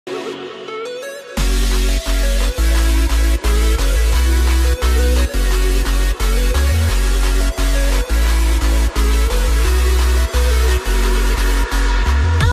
Skiing short turns with a sunset
A few short turns with a sunset in tirol.